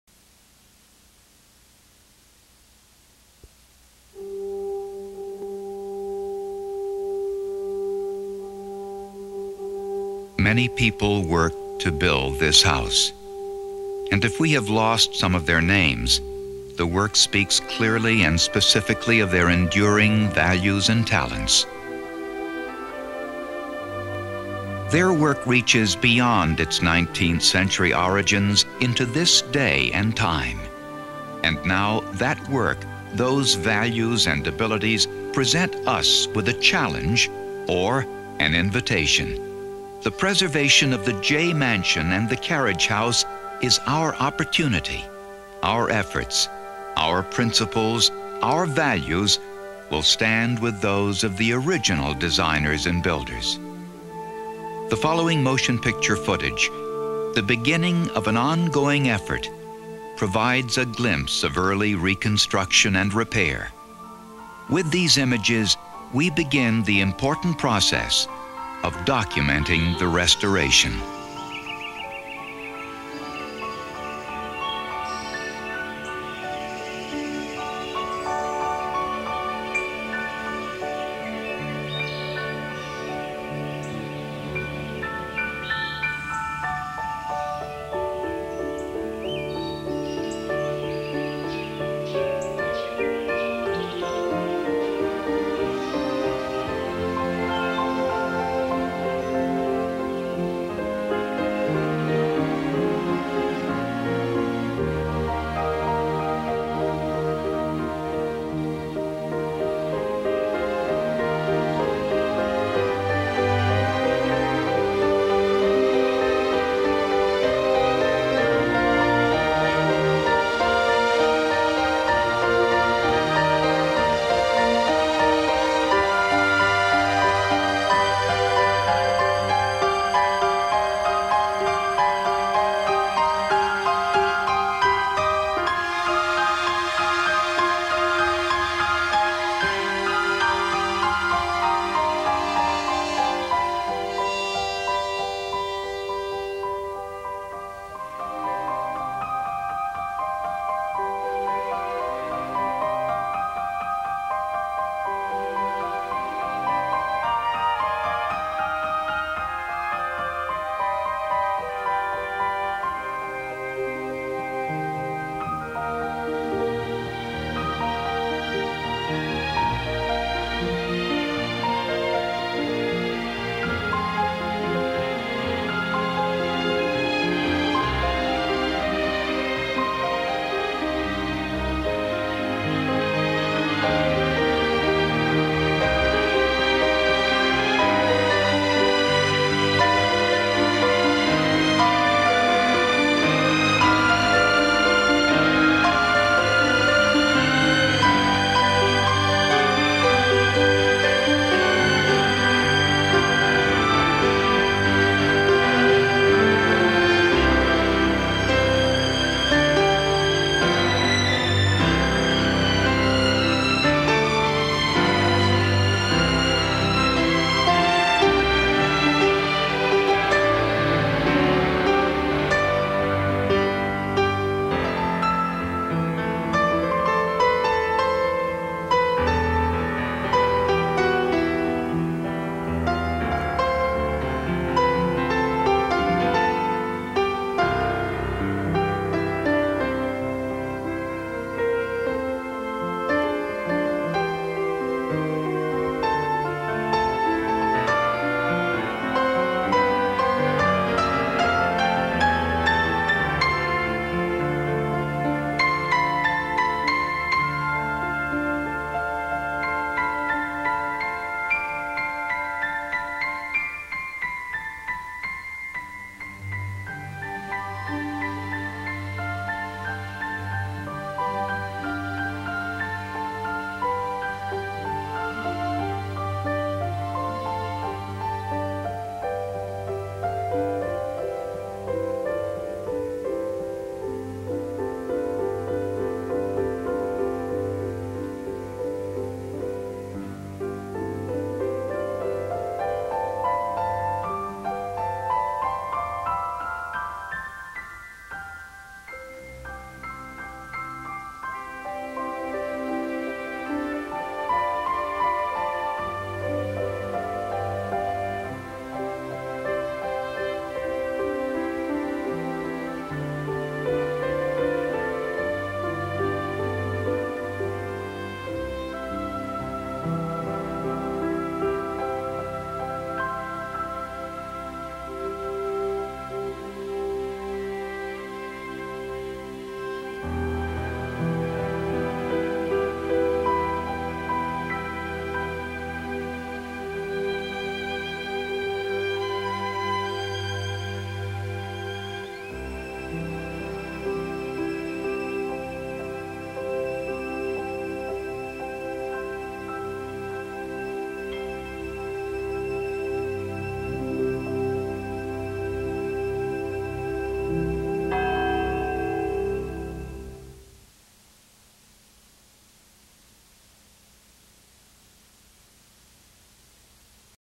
Jay Heritage Center - Stabilizing the 1838 Jay Mansion and the 1907 Van Norden Carriage House
Video footage of the Jay Mansion circa 1993.

In the early 1980s and 1990s, a coalition of 62 historic and environmental organizations, led by the founders of today's non-profit Jay Heritage Center fought a valiant and successful 13 year battle to save the boyhood home and landscape of Founding Father, John Jay, from commercial development and destruction. Their efforts helped protect our American heritage, as well as the adjacent Marshlands Conservancy and Long Island Sound; their efforts also preserved the elegant 1838 Peter Augustus Jay mansion built by John Jay's eldest son on the exact site of his father's home, "The Locusts." This video shows the initial steps taken to stabilize the 1838 Jay Mansion -- it's soaring Greek Revival columns and capitals were carefully removed and restored --and the 1907 Van Norden Carriage House--whose original clock was restored to working order.

Isalnd Housewrights, a Staten Island Company was assigned to take down and restore the columns of the mansion while Ashwood Restoration worked on the portico itself. The columns took about 10 months to repair and were returned in July 1994.

(Video Credit: Von Spaeth Productions for the Jay Heritage Center)

To see photos of the house today visit us on Flickr at